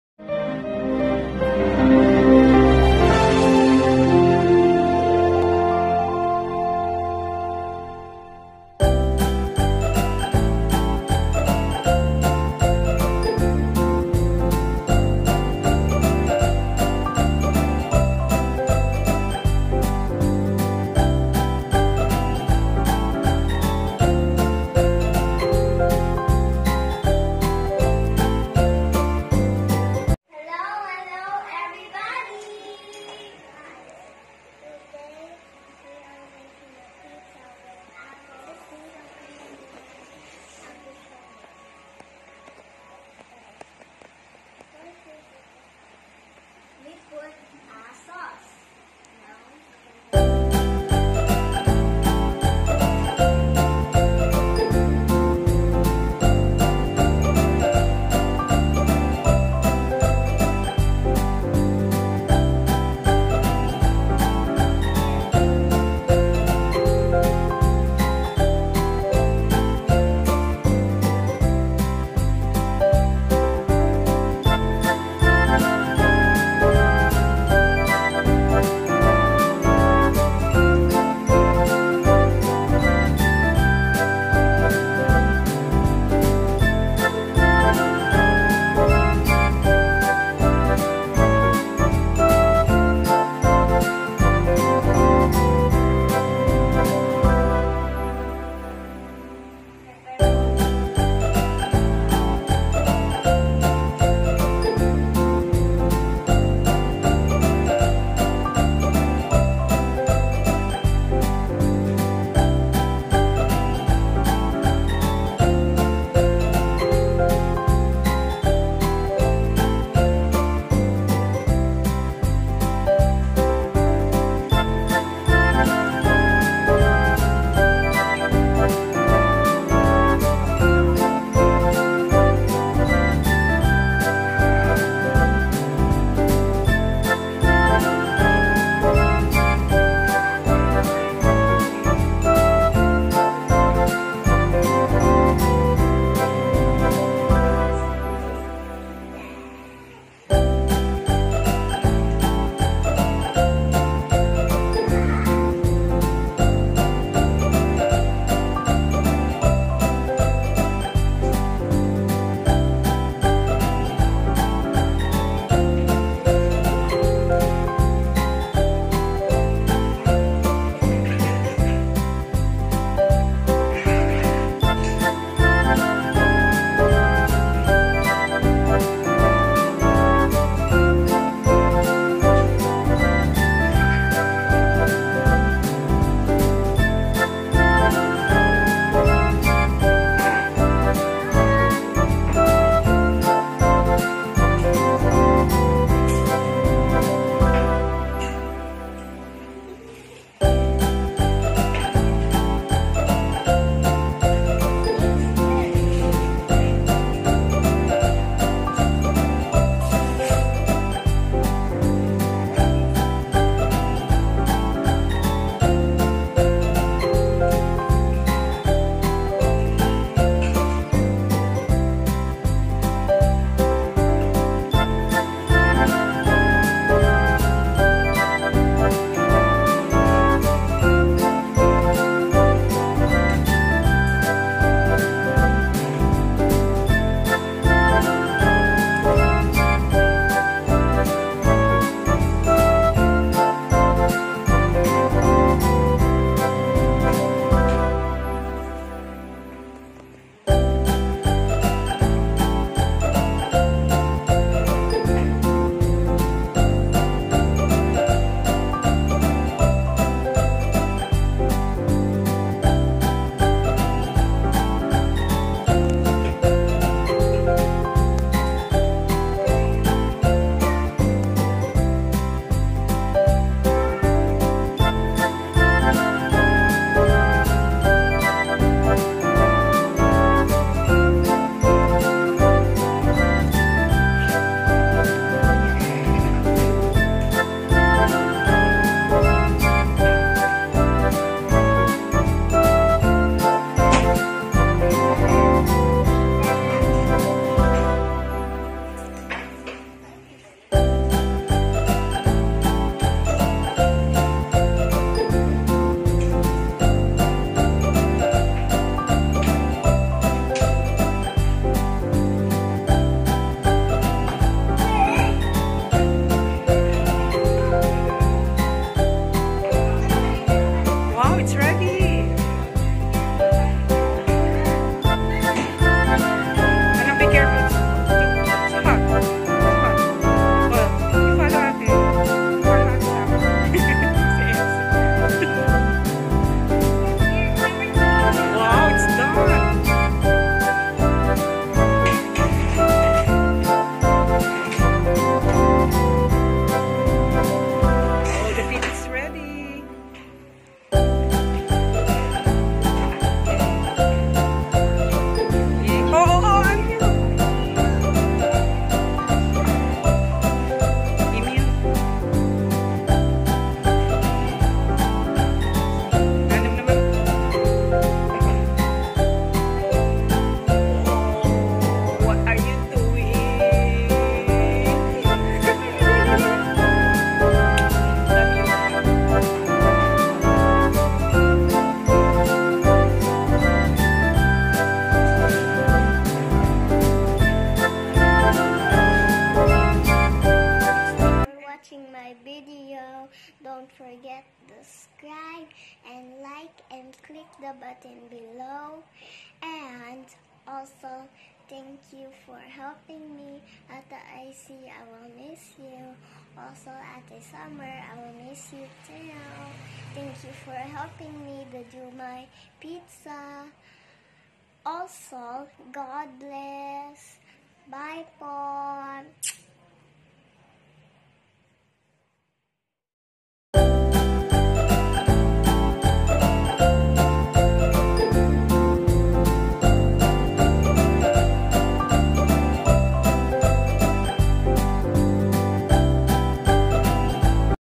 PIZZA TIME WITH FRIENDS//#005
A simple bonding, a gathering of friends that Ammiel didn't realize she was making memories. She just knew she was having fun.
Ammiel, in this video is making pizza with her Ate Icy and Ate Summer and it was really a delicious pizza.